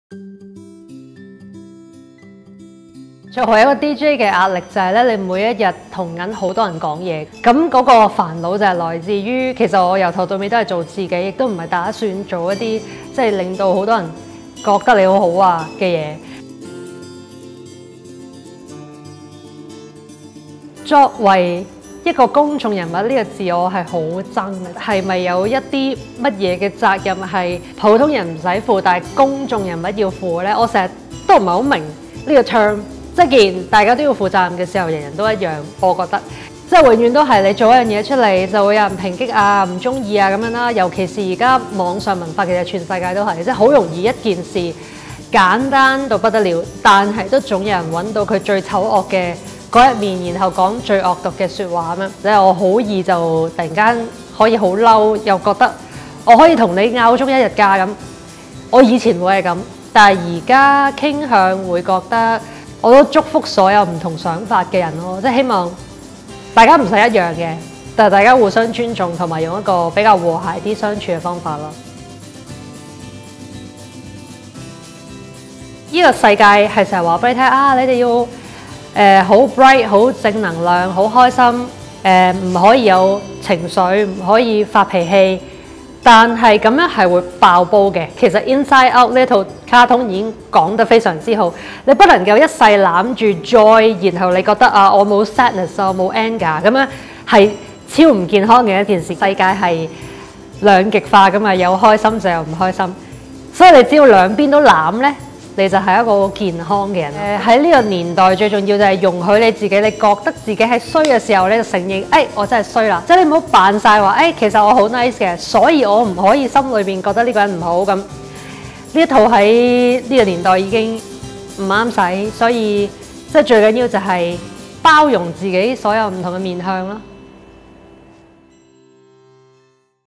麻利：承認自己有壞的一面才是最健康 | Harper's BAZAAR TV
電台節目主持人麻利，一直都沒有想過要刻意討好聽眾，從來都在做自己，但面對現今網絡的文化，就算發佈一件簡單小事，都可以被無限放大醜化，甚至惡意攻擊，對於以前的她會與網民爭論，但自從了解身心靈層面的健康觀後，她作出了這些外變...。